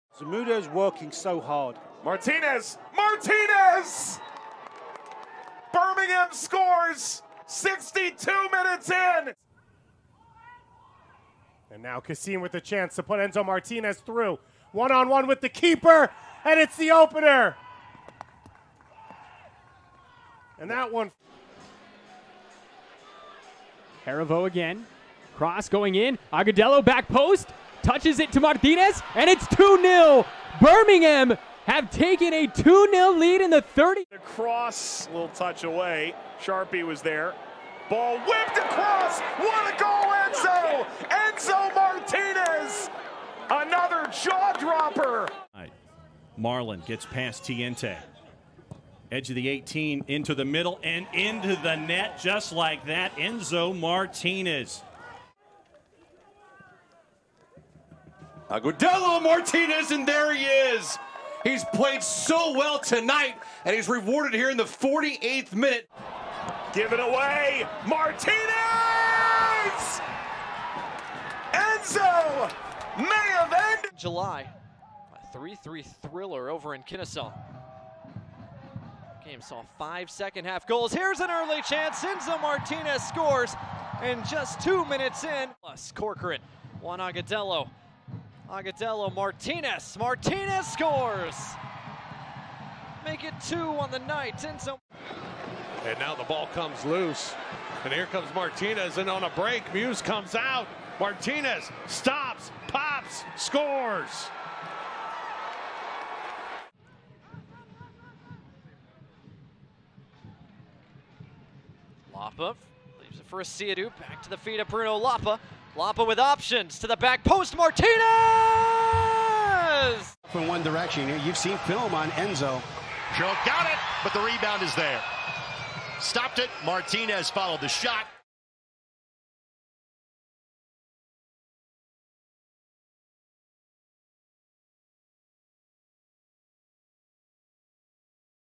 Enzo Martinez: Goal-Scoring Machine
(9/12/22) All goals for Enzo Martinez, top-scorer for Birmingham Legion FC with twelve goals to his name.